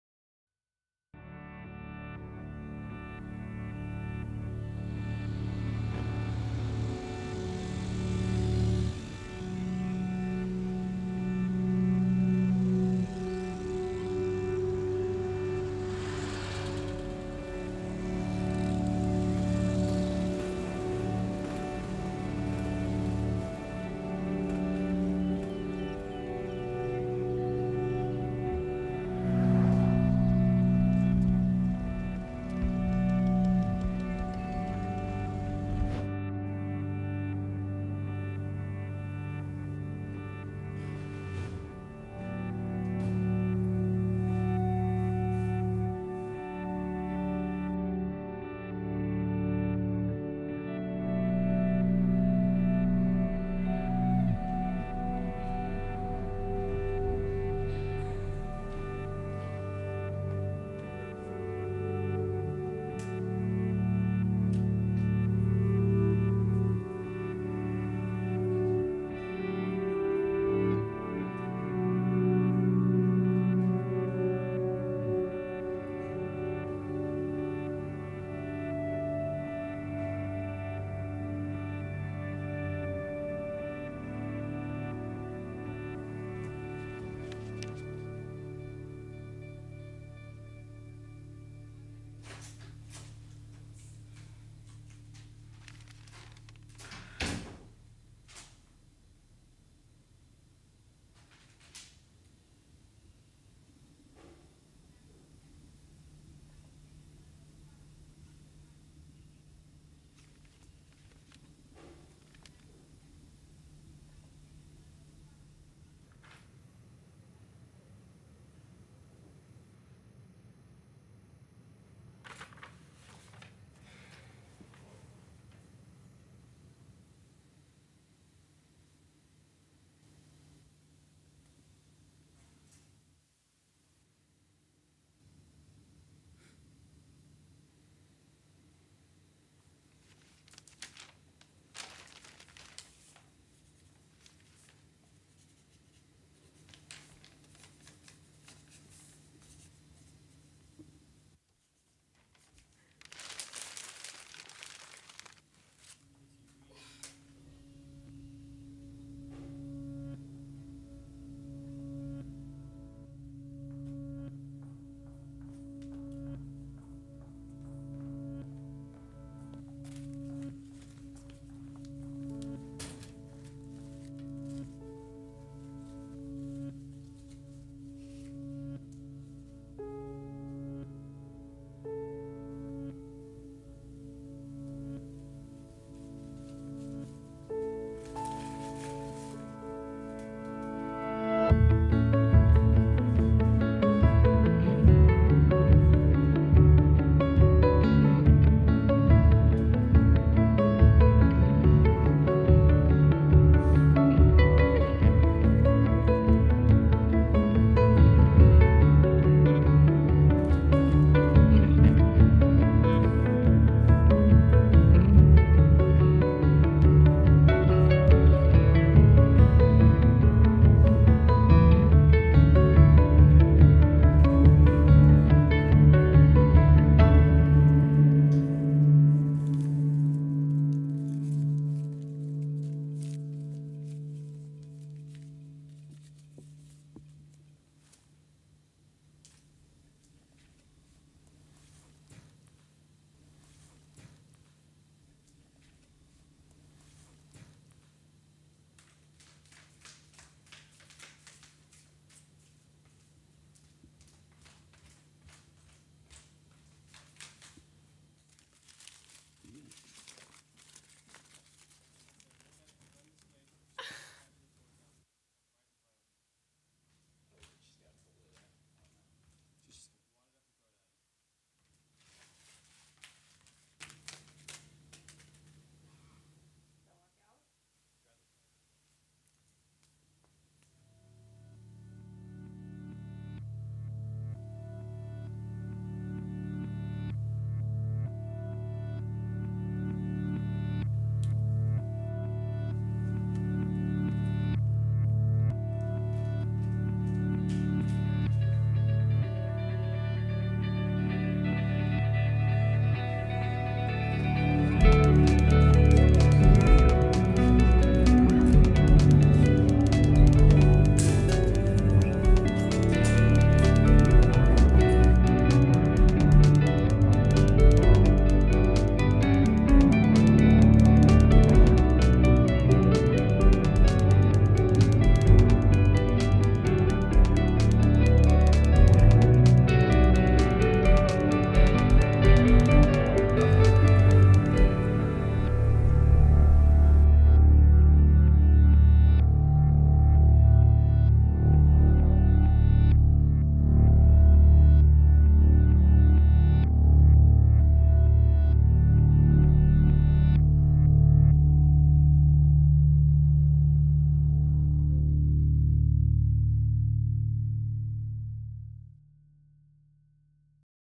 Love Story
WON A GOLD KEY AT THE NATIONAL SCHOLASTICS AWARDS! Little love story produced by Purple Phlow Productions via Westlake High School. Filmed with an HV 30, edited in Final Cut Pro, music written/recorded by James Lambrecht and Gabe Sandoval at Tequila Mockingbird studios in Austin, Tx.

thank you TK, Sarah, Paul, Ben, John, Gabe, Wally, Mrs. Burdett, Mrs. Abney, Shelly.